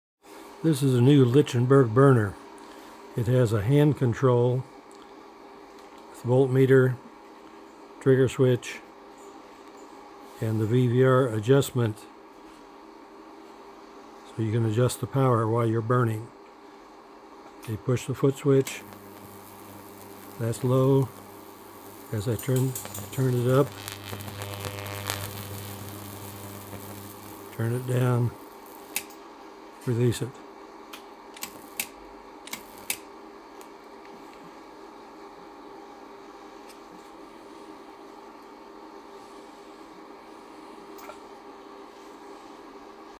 Lichtenberg Wood Burner that you can buy, ready to use. Safety First!
Join our Facebook group: "Lichtenberg Wood Burning Safety".

More than 600 people worldwide are enjoying this hobby with a burner that is reliable and safer than any burner you will see on YouTube.

My Burner Features

My burner is safer than most DIY YouTube burners because:
1. The transformer that is safer than a MOT from a microwave oven. A MOT is not designed to be used to burn wood. The MOT steel core is one leg of the HV circuit, very dangerous!
2. A thermo-switch on the secondary coil opens if the transformer gets over 130C degrees.
3. 20A power cord,
4. 30A manual reset CB,
5. Lighted power switch,
6. Dedicated burner switch,
7. Isolated control circuit,
8. 30A power relay,
9. 12AWG silicone power wiring, 18AWG control circuit
10. Plug-in footswitch,
11. Optional hand controller,
12. Double insulated HV 80" leads,
13. PVC probes with ceramic insulators, 30KV disconnects.
14. Earth-grounded transformer, and aluminum chassis,
15. Volt/Ammeter,
16. 5000-watt VVR (Variable Voltage Regulator) to limit power for finely detailed burns, (Same purpose as a Variac).
17. Two push-pull ventilation fans,
18. LEDs to indicate that the power is "ON",
My burners were not designed on YouTube, they were designed because of YouTubes' dangerous videos.
NOTES:
An unsafe YouTube burner is cheap to make and very easy by YouTube videos. The downside is that you may not live to enjoy the art that you produce!
I can’t design a “safe-to-use” burner without using many quality components.